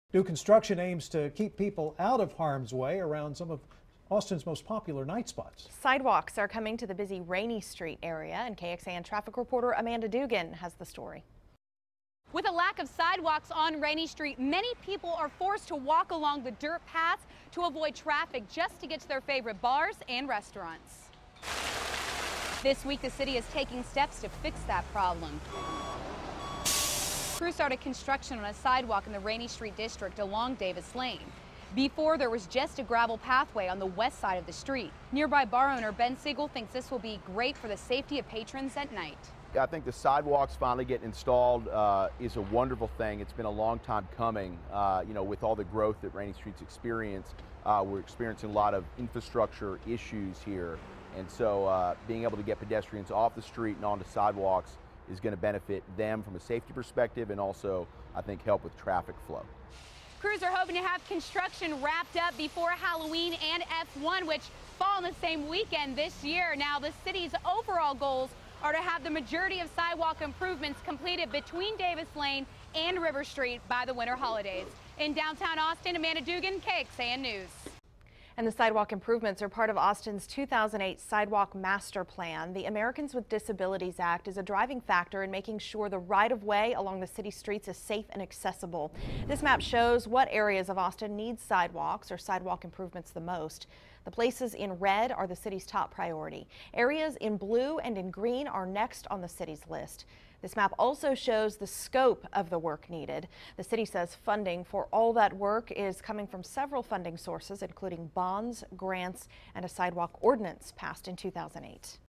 Rainey Street sidewalk construction begins
The lack of sidewalks on Austin’s historic Rainey Street forces pedestrians to walk on the dirt in order to avoid traffic.